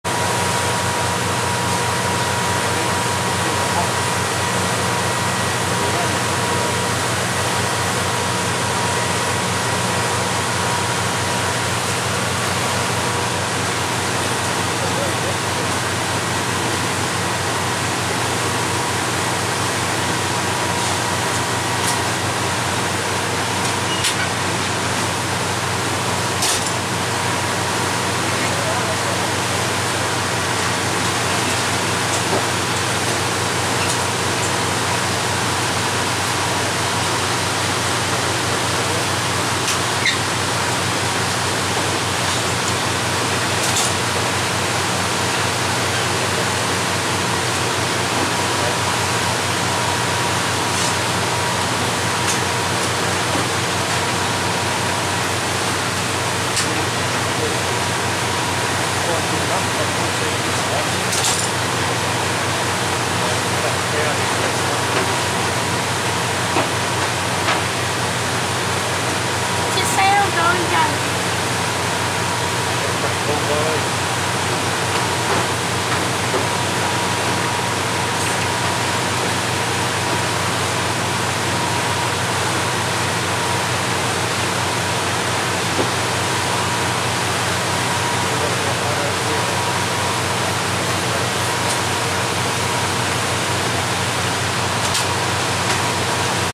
Amazing scenes in Milford Sound, New Zealand
Milford Sound rain causes waterfalls from everywhere. Milford Sound is the wettest inhabited place in New Zealand, and one of the wettest places in the world! The mean annual rainfall in Milford Sound is 6,813mm or 268 inches, and it rains there on average 182 days a year. All that rain has to go somewhere and I was lucky enough to time it during a heavy patch, it was predicted up to 25mm (1 inch) per hour and a total of +75mm in 12 hours.